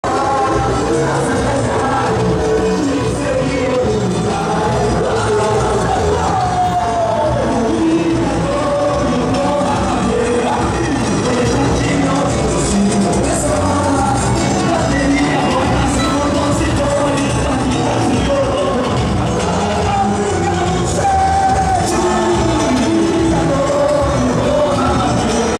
優勝チーム サウージ 道路に落ちたゴミを拾う 浅草サンバカーニバル 2015 Asakusa samba carnival 2015！
2-012　サウージ　浅草サンバカーニバル　2015
道路に落ちたゴミを拾う！
Asakusa samba carnival 2015
浅草サンバカーニバル　2015　サウーヂ
エスコーラ・ヂ・サンバ・サウーヂ
（エスコーラ・ヂ・サンバ・サウーヂ） 29回 1986年
神奈川県横浜市野毛

16:55 スタート

浅草サンバカーニバル　　サウーヂ　2015